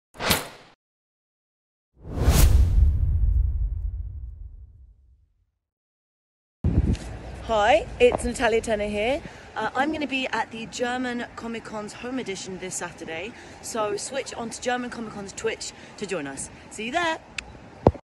Natalia Tena @ GCC Home Edition Live Signing 21.11.2020
Seid dabei und schaltet ein zu unserem zweiten ONLINE LIVE SIGNING am 21. November 2020 auf Twitch!